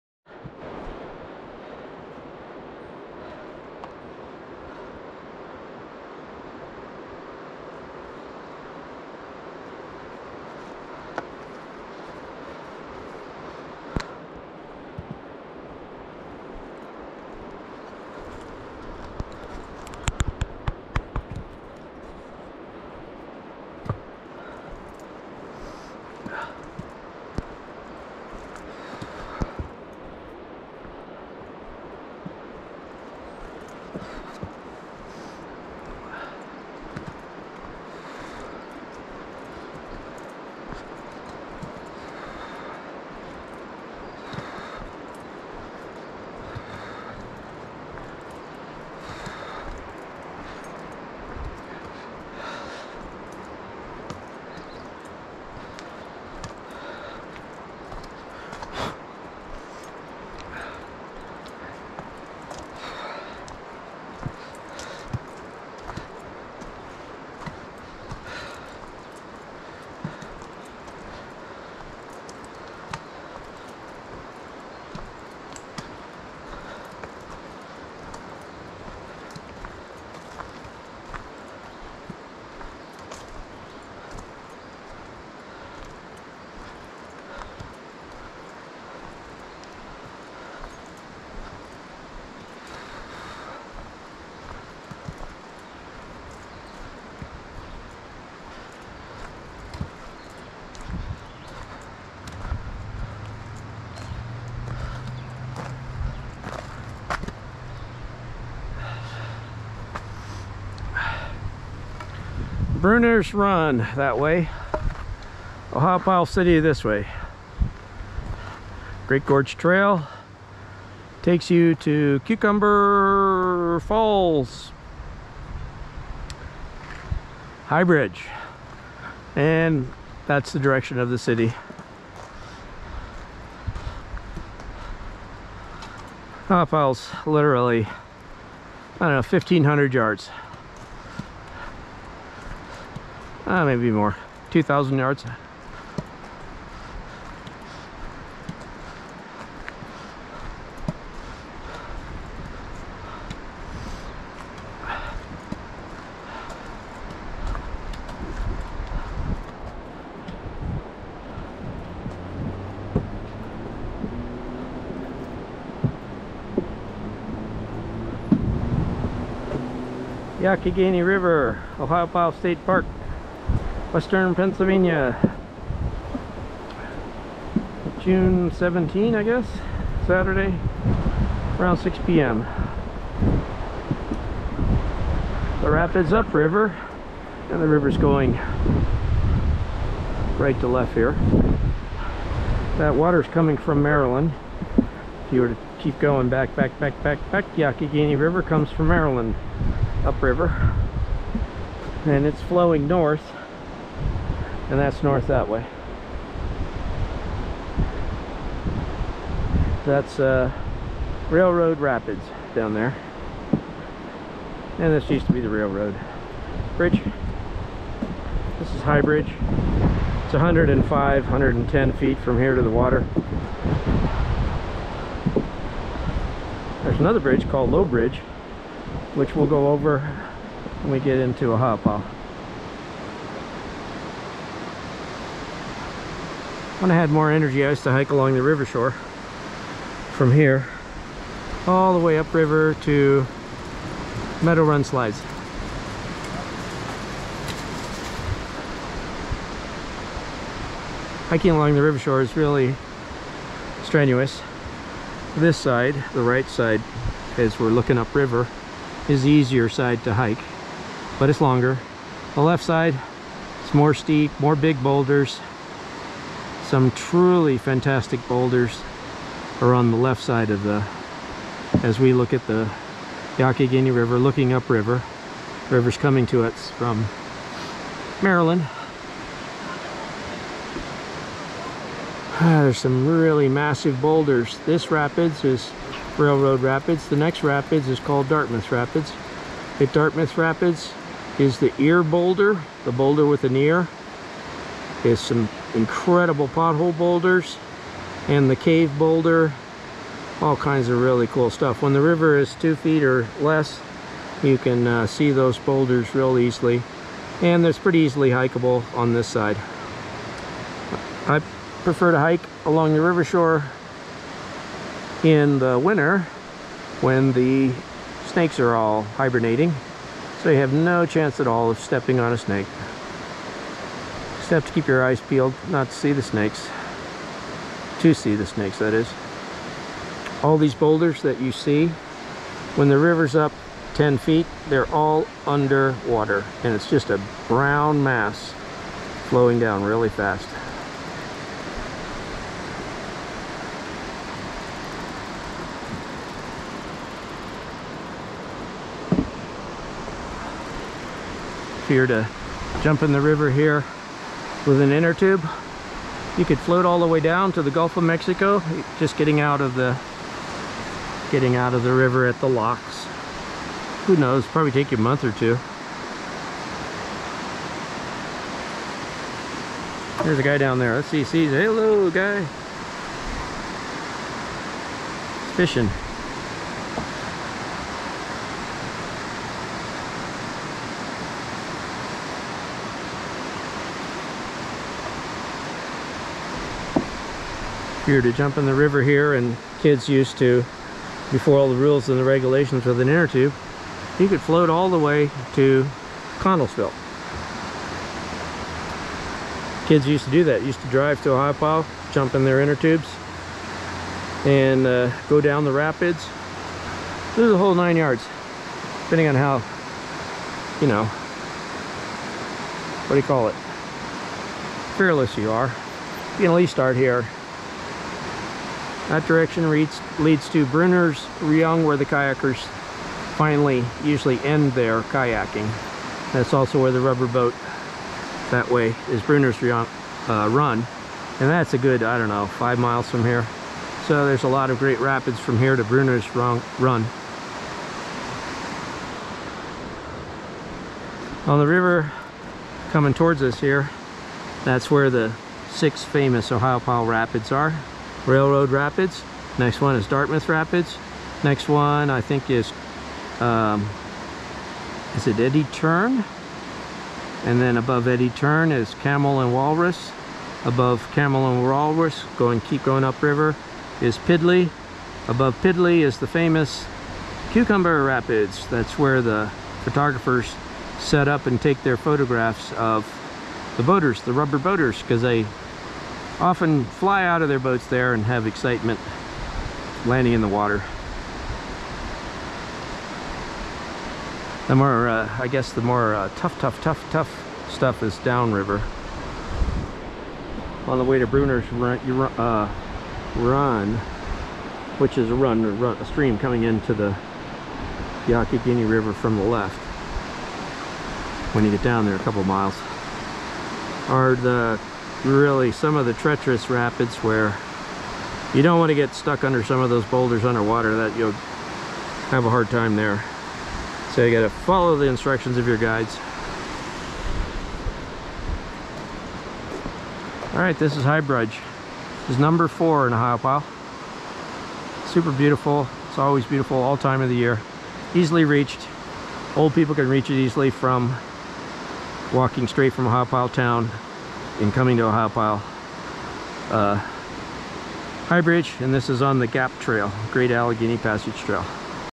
Ohiopyle 17 June 2023 Great Gorge Trail coming to High Bridge
Coming to the end of the Great Gorge trail, then cross High Bridge, heading towards Ohiopyle city.

You can see Railroad Raids upriver from High Bridge.